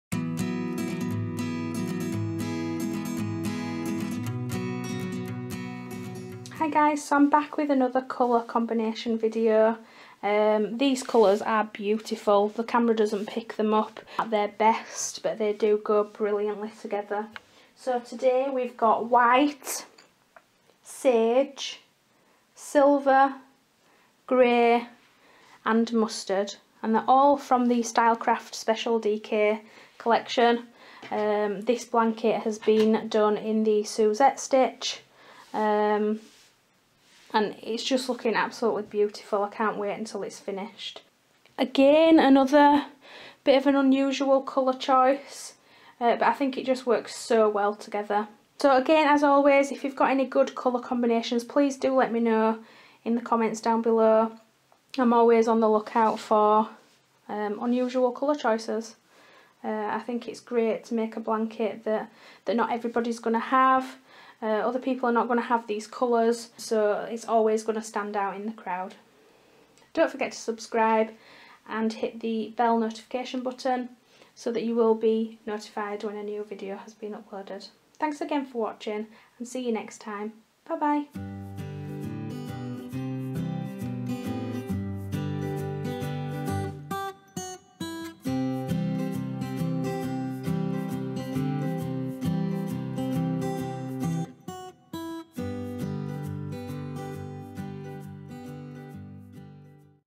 YARN COLOUR COMBOS: Stylecraft Special DK Colour Combination #2 | Nohella Jayne Designs
Yarn - Stylecraft Special DK in White, Grey, Silver, Sage and Mustard.

Please leave your suggestions in the comments section of videos you would like to see in the future.

SJ xx